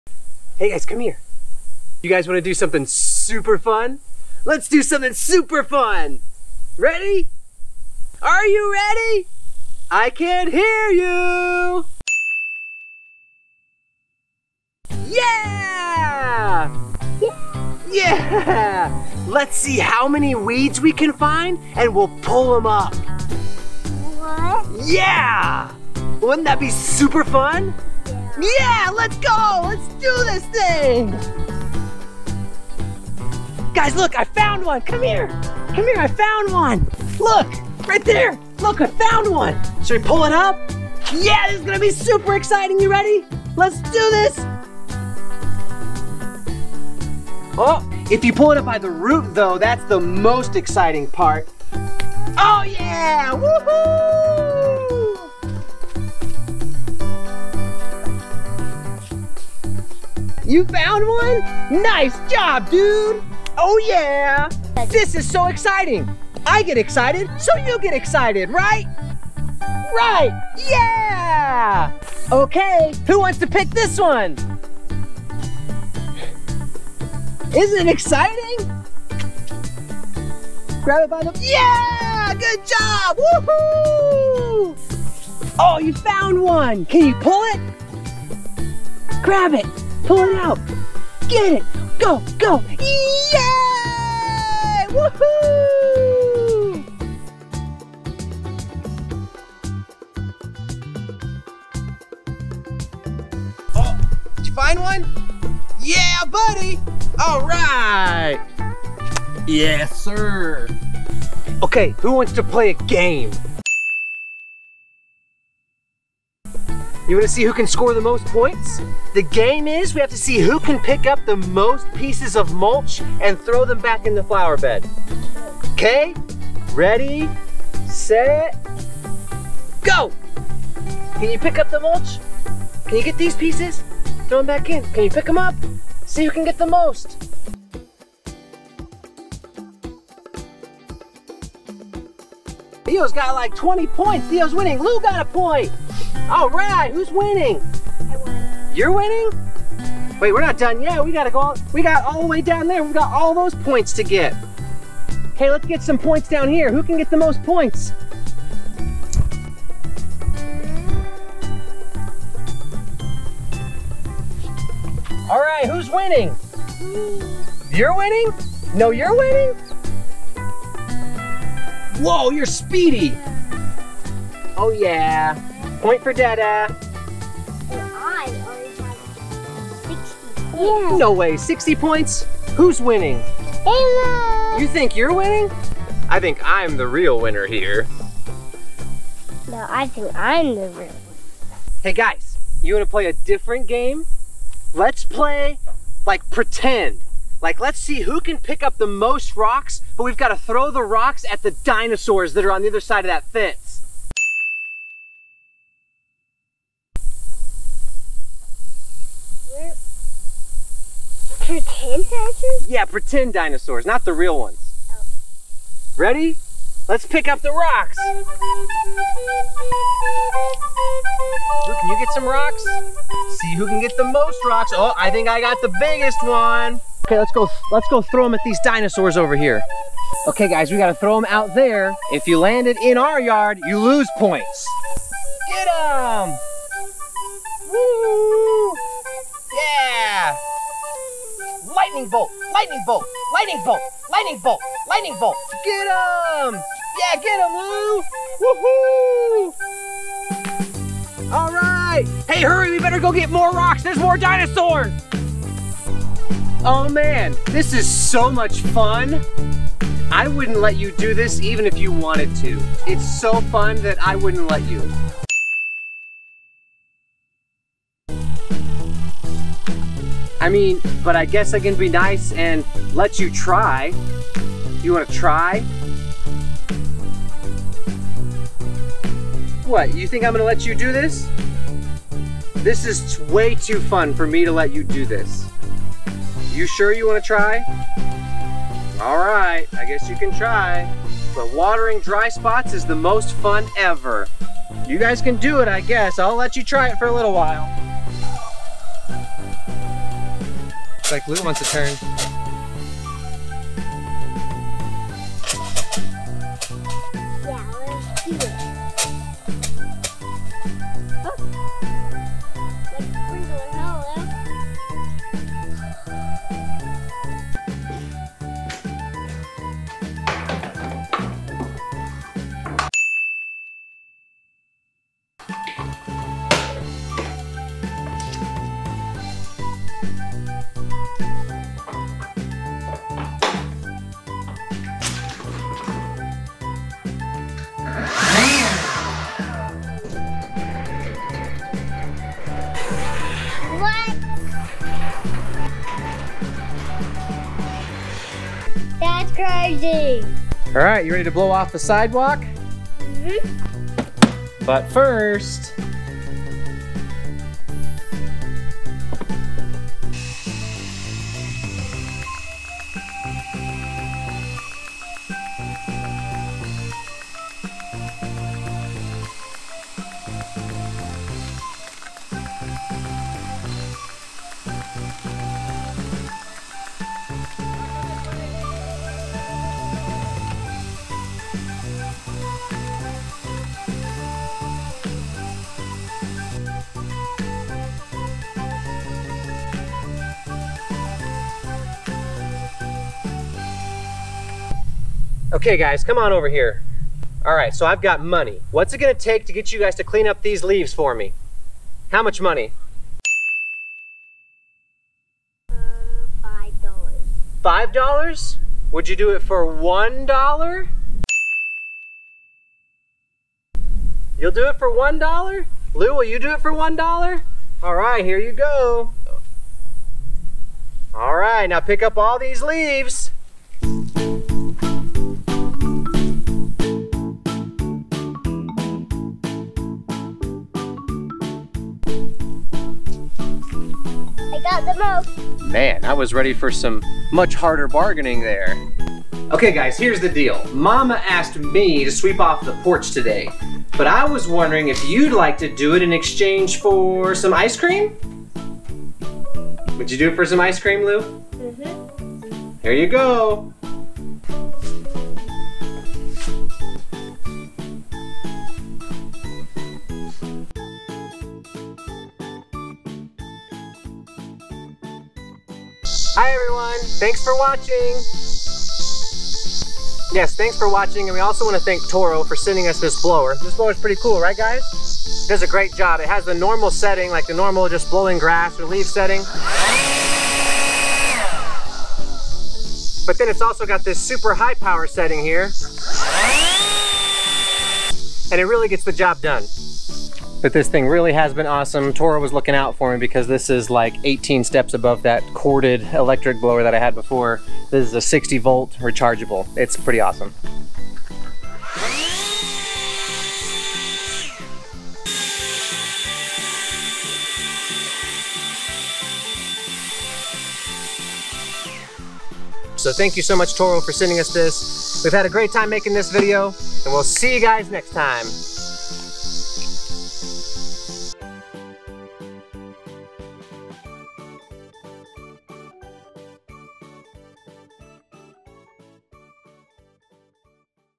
6 SECRETS To Getting Your Kids To Help With Lawn Work
I’ve discovered the secret to getting a premium lawn with half the effort! Can’t wait for the kids to get older and move them in to mowing, weed eating and more! Combining my favorite things, being with my family and yard care.

Thanks again to Toro for sending the 60v blower to us!